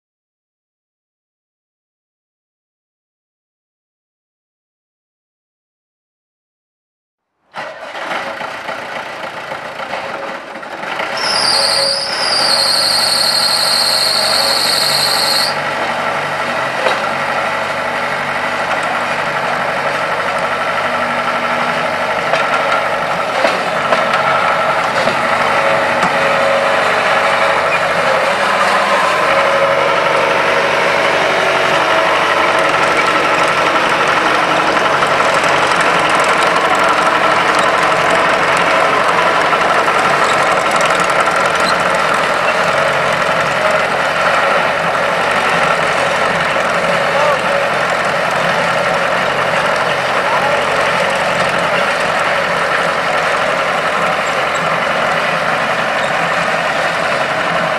Ditch Witch 4500 trencher for sale | sold at auction May 28, 2015
Ditch Witch 4500 trencher
8 hours on meter
Deutz FL1011E diesel engine
45 HP
Serial 7783543
Hi-Lo gears
Four wheel drive
ROPS
H412 hydraulic slide trencher
90" boom
Auxiliary hydraulics
65"W x 14"H back fill blade
29 x 12.5-15 tires
Serial 3N1324